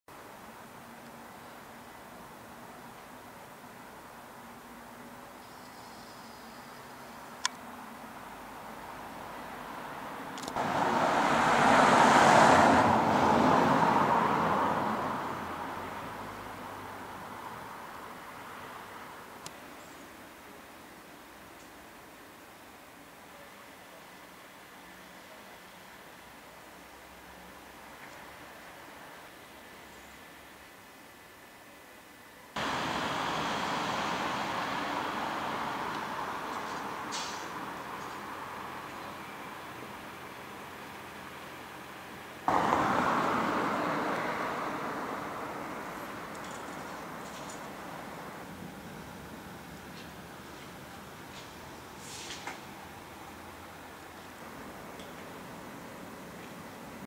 Byty v Komárově
Budování spousty nových bytů v Brně Komárově aktuálně.